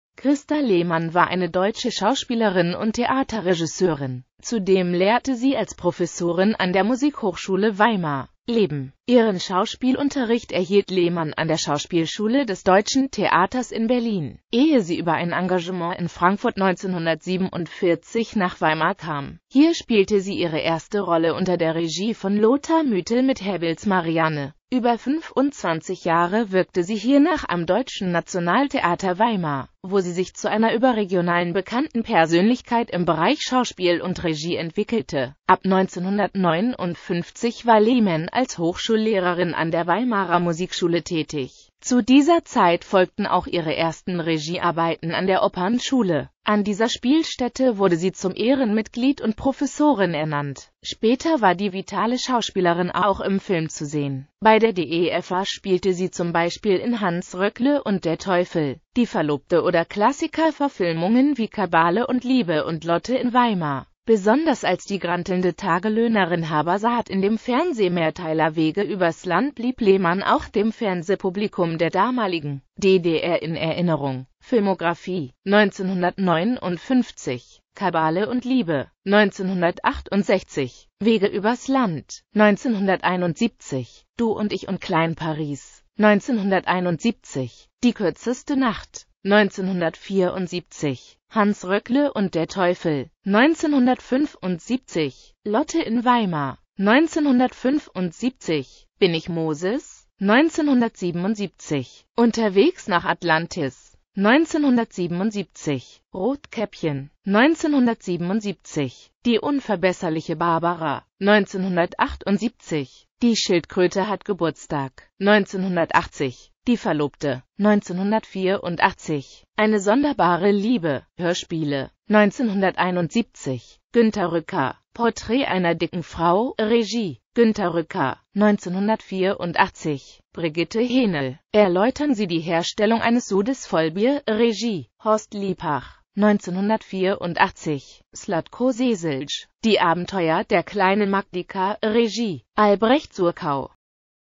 Christa Lehmann (Schauspielerin)
Vielen Dank für Ihre Unterstützung:

Christa Lehmann (* 30.März 1921; † 18.Oktober 1992 in Weimar) war eine deutsche Schauspielerin und Theaterregisseurin.Zudem lehrte sie als Professorin an der Musikhochschule Weimar.